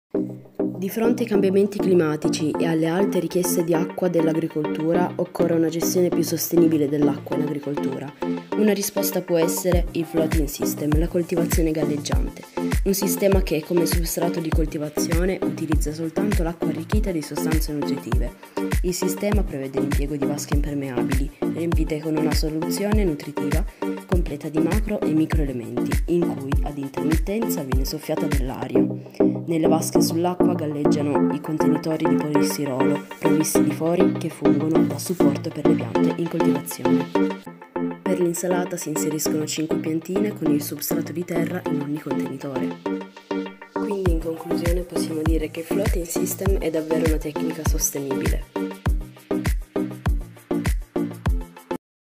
2° Classificato contest Istituti Agrari: Istituto Superiore “B. Vittone” - Classe 4C
Floating System

L’acqua, bene essenziale per l’attività agricola. Ma come fronteggiare la crescente richiesta idrica e la necessaria razionalizzazione della risorsa di fronte all’emergenza climatica?
La classe 4C dell’Istituto Superiore “B. Vittone”, 2° classificato del contest Istituti Agrari, offre una soluzione efficace: la coltivazione galleggiante. Un sistema che permette di massimizzare l’utilizzo dell’acqua senza sprecarla.